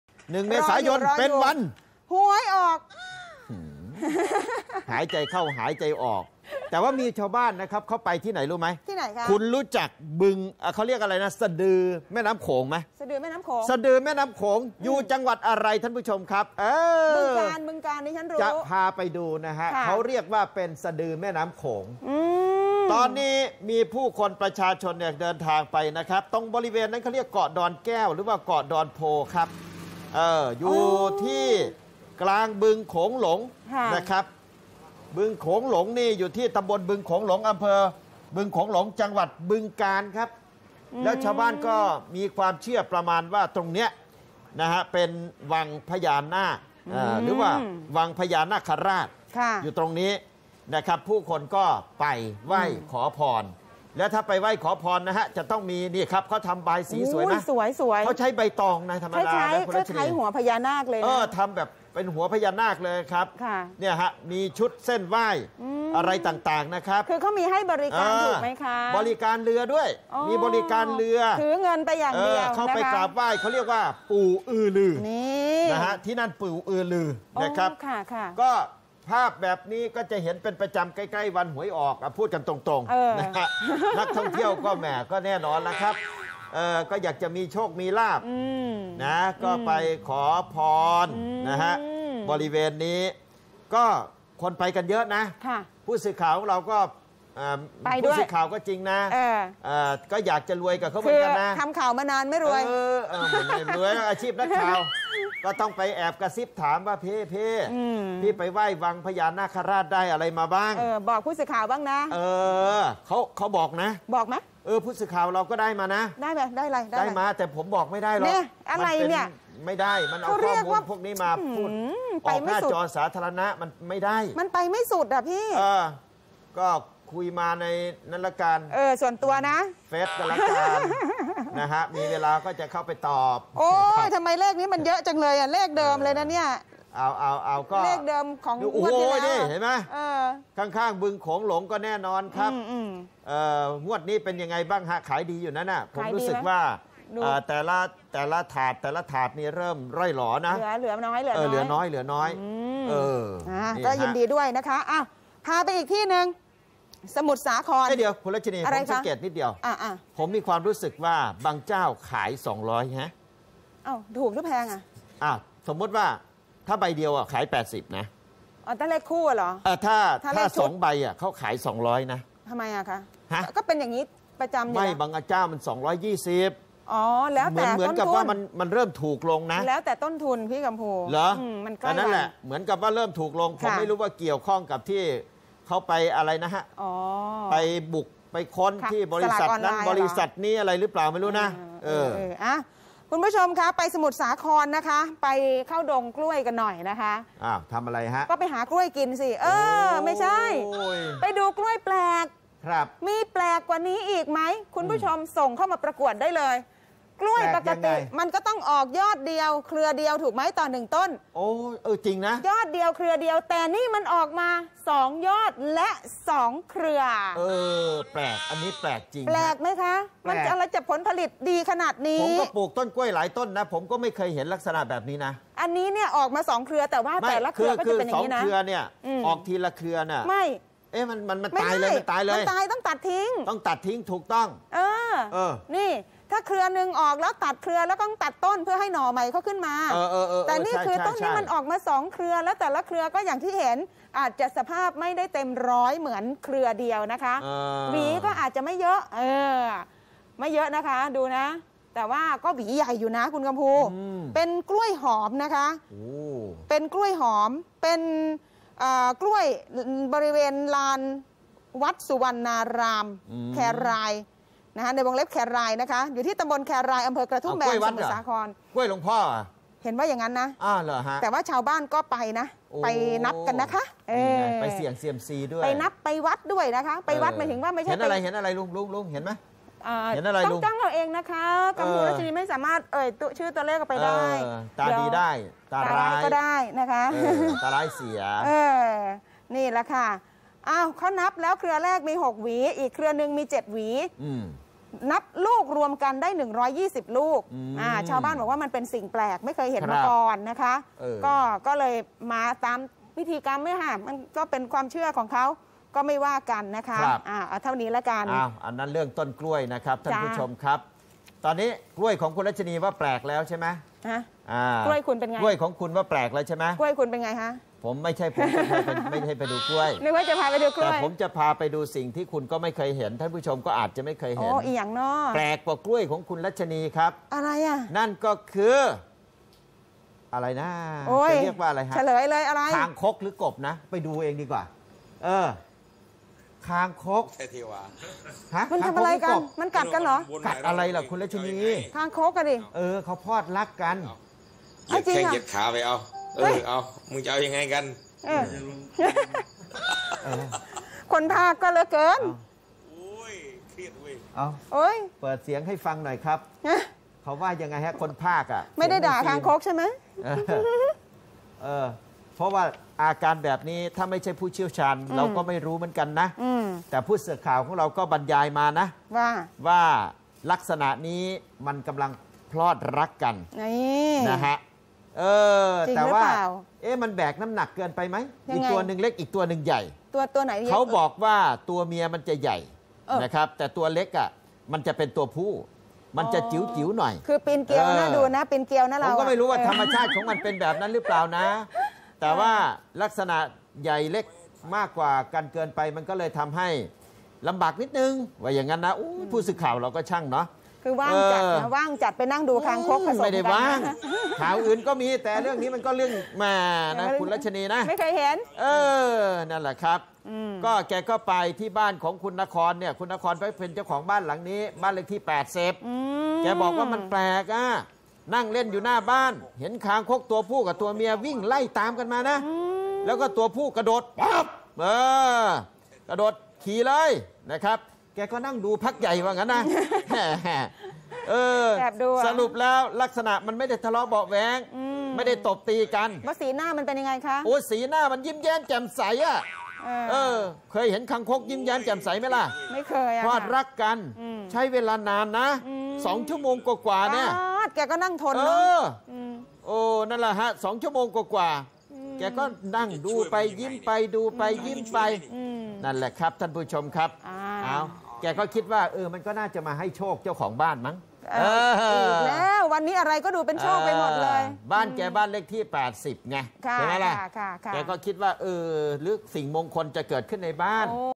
สีสันก่อนวันหวยออก หลายจังหวัดแห่ขอเลขคึกคัก
ที่บริเวณเกาะดอนแก้ว เกาะดอนโพธิ์ มีนักท่องเที่ยวทยอยนั่งเรือ เข้าไปกราบไหว้ปู่อือลือ สิ่งศักดิ์สิทธิ์ที่อยู่บนเกาะกลางบึงโขงหลงแห่งนี้ เพื่อขอพรสิ่งศักดิ์สิทธิ์